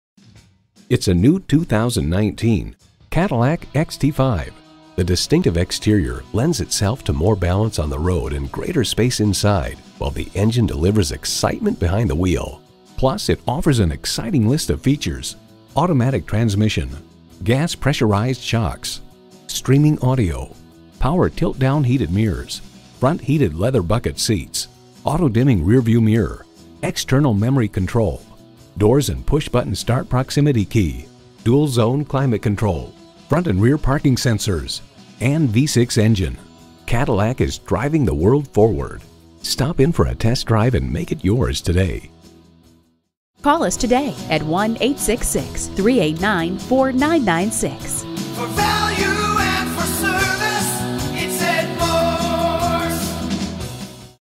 New 2019 Cadillac XT5 Miami Fort Lauderdale, FL KZ133282 - SOLD
Year: 2019
Make: Cadillac
Model: XT5
Trim: Luxury
Engine: 3.6l 6 Cylinder Fuel Injected
Transmission: FWD 8-Speed
Color: Black Metallic
Interior: Beige
Stock #: KZ133282
VIN: 1GYKNCRS7KZ133282

Address:
1240 North Federal Highway